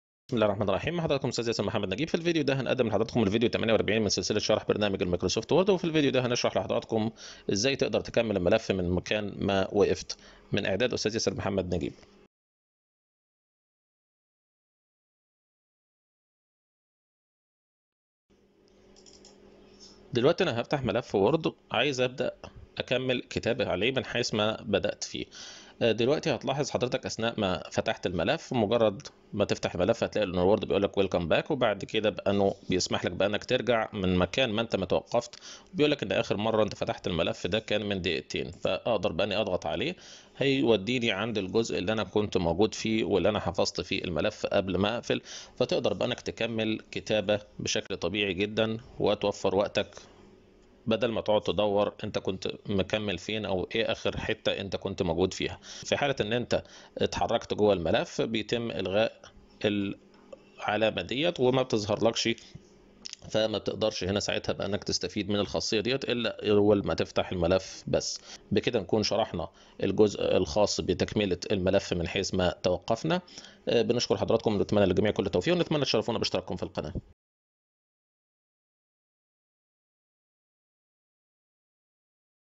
MS Word (48) - تكملة الملف من حيث توقفت
في هذا الفيديو نقدم لكيفية تكملة الملف من حيث توقفت .